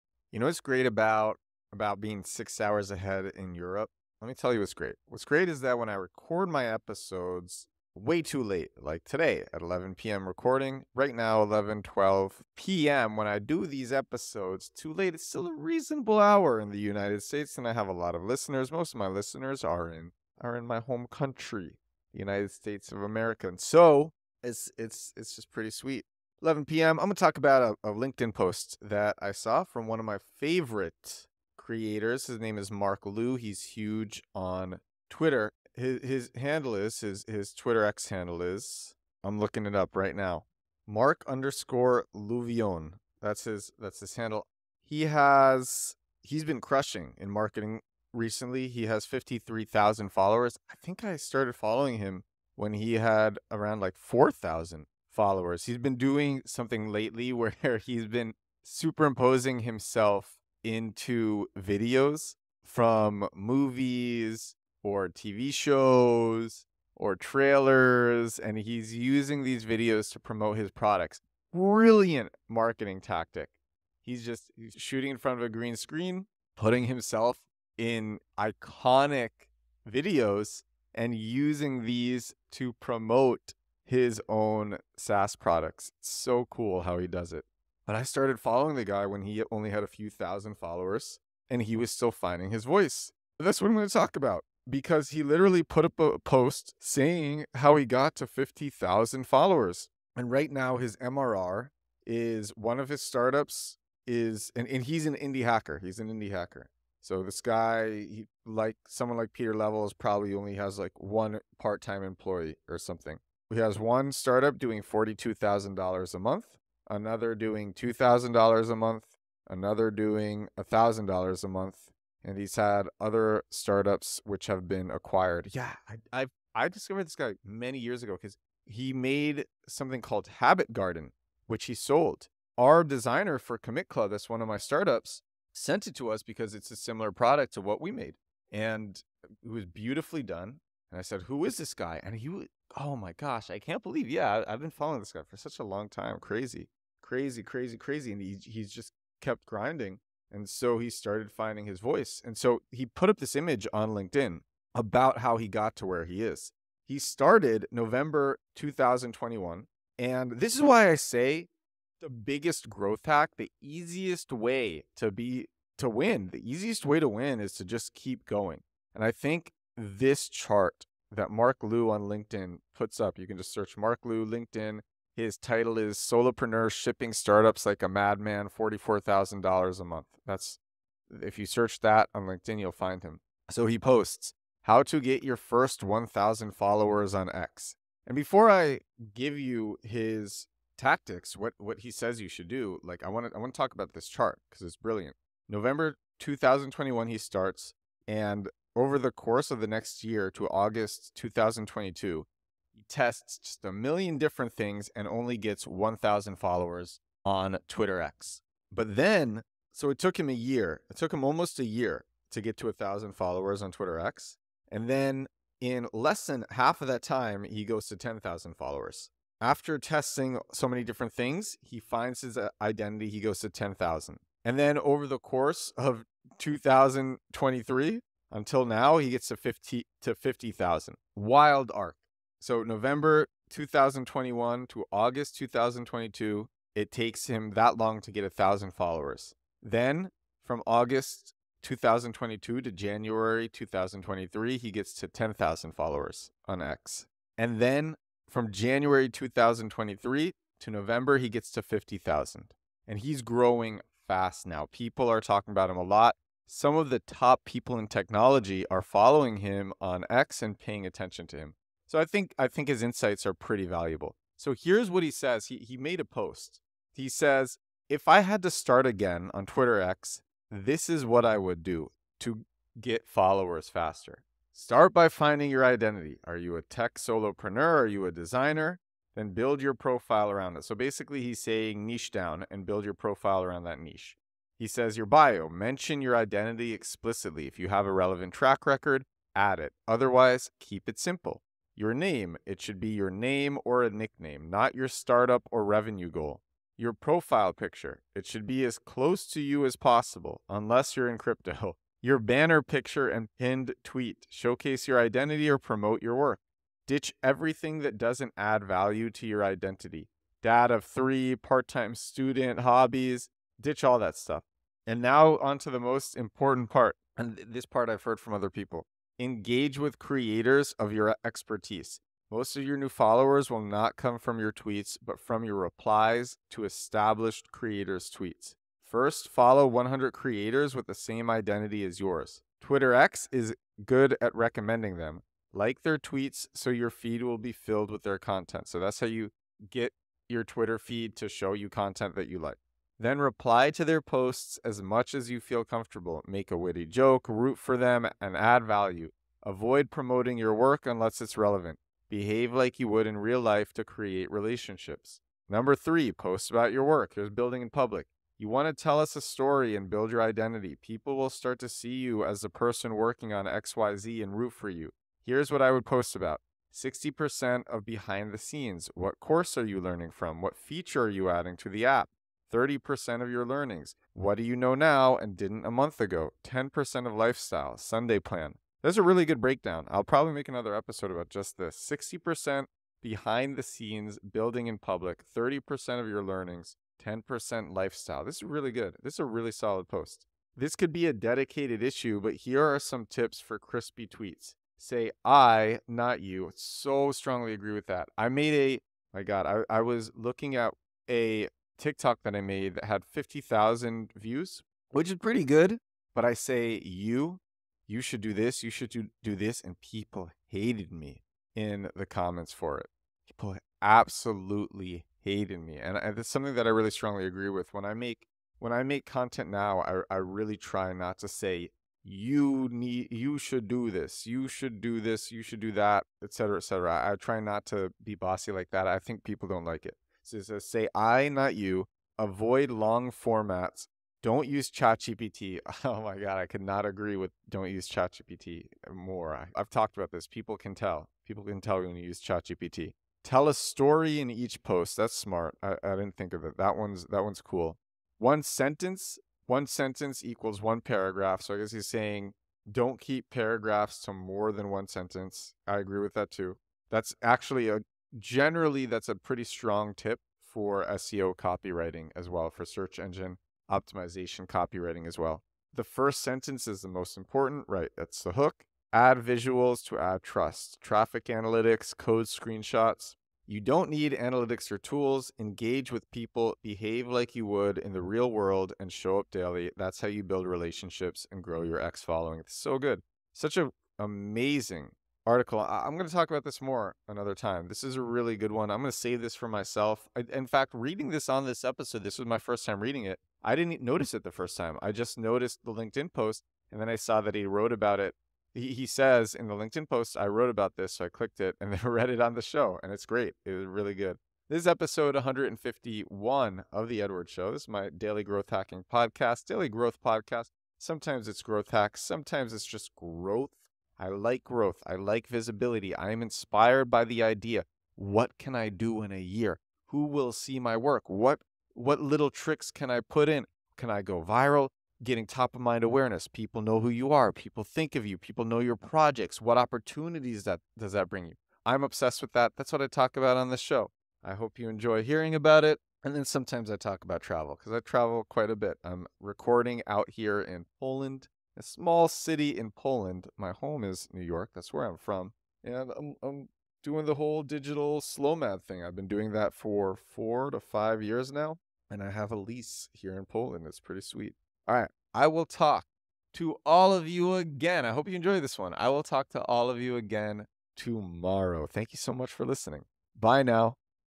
How to Get Your First 1,000 Followers on 𝕏 (#151)
E151: If you want to grow on TwitterX by doing Build in Public content - or grow on TwitterX in the startup and technology entrepreneurship niche - this episode will give a framework to do so.

The episode is about Marc Lou's "How to get your first 1,000 followers on TwitterX" article.

Marc shares what he would do to shortcut his follower growth if he were starting again on TwitterX.

His insights are short and impactful.